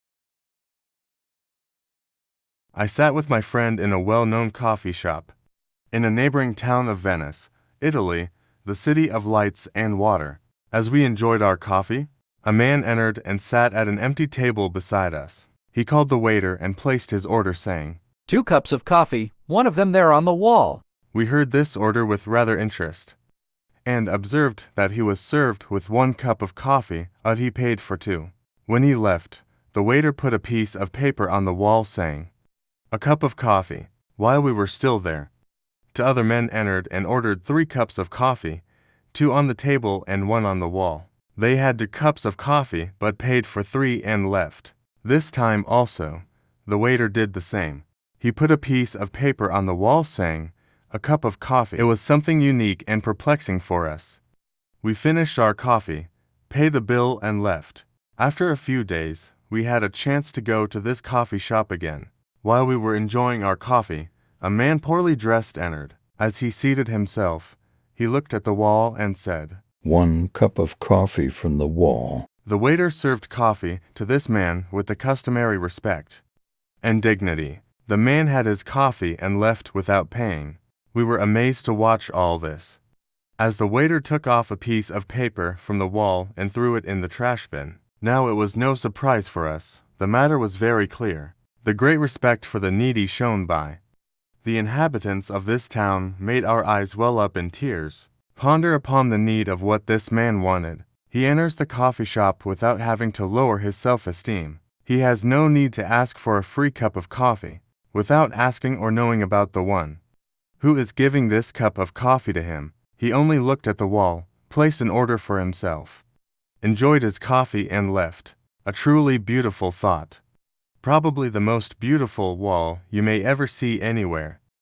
Coffee on the Wall | Short Story
I sat with my friend in a well-known coffee shop in a neighboring town of Venice, Italy, the city of lights and water.

As we enjoyed our coffee, a man entered and sat at an empty table beside us. He called the waiter and placed his order saying, “Two cups of coffee, one of them there on the wall.”

We heard this order with rather interest and observed that he was served with one cup of coffee but he paid for two.

When he left, the waiter put a piece of paper on the wall saying “A Cup of Coffee”.

While we were still there, two other men entered and ordered three cups of coffee, two on the table and one on the wall. They had two cups of coffee but paid for three and left. This time also, the waiter did the same; he put a piece of paper on the wall saying, “A Cup of Coffee”.

It was something unique and perplexing for us. We finished our coffee, paid the bill and left.

After a few days, we had a chance to go to this coffee shop again. While we were enjoying our coffee, a man poorly dressed entered. As he seated himself, he looked at the wall and said, “One cup of coffee from the wall.”

The waiter served coffee to this man with the customary respect and dignity. The man had his coffee and left without paying.

We were amazed to watch all this, as the waiter took off a piece of paper from the wall and threw it in the trash bin.

Now it was no surprise for us – the matter was very clear. The great respect for the needy shown by the inhabitants of this town made our eyes well up in tears.

Ponder upon the need of what this man wanted. He enters the coffee shop without having to lower his self-esteem… he has no need to ask for a free cup of coffee… without asking or knowing about the one who is giving this cup of coffee to him… he only looked at the wall, placed an order for himself, enjoyed his coffee and left.

A truly beautiful thought. Probably the most beautiful wall you may ever see anywhere!